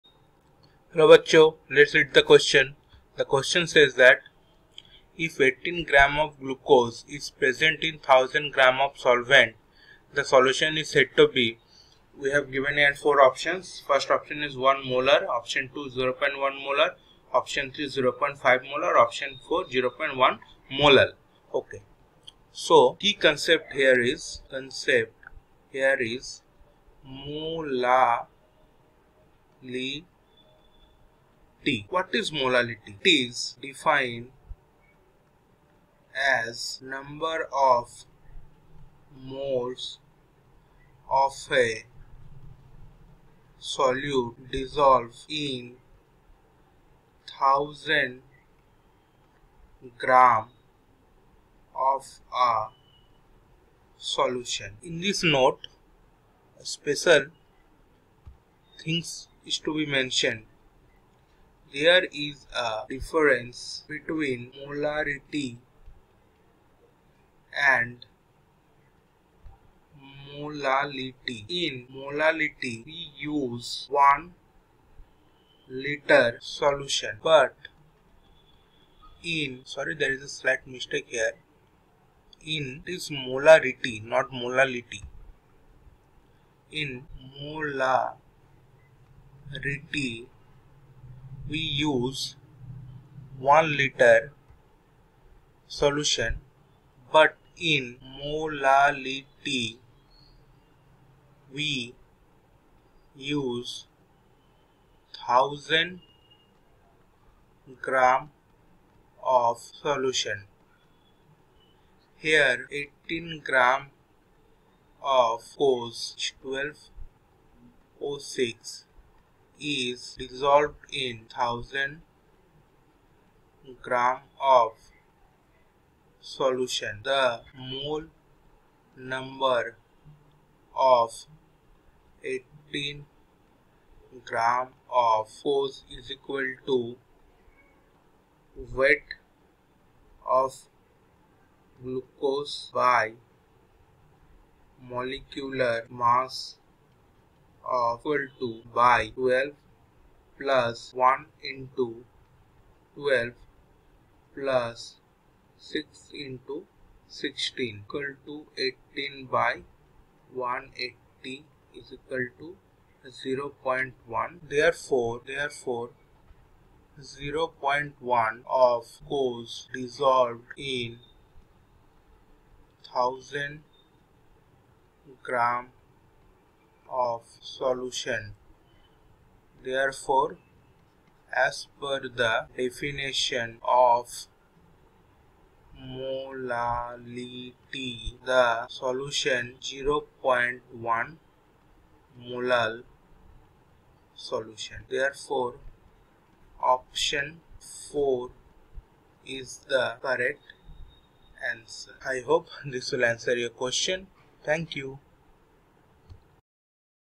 , If 18 g of glucose is present in 1000 g of solvent, the solution is said to be : IV (1) 1 mol...
, If 18  g of glucose is present in 1000  g of solvent, the solution is said to be :
IV
(1) 1 molar
(2) 0.1 molar
(3) 0.5 molar
(4) 0.1 molal, ,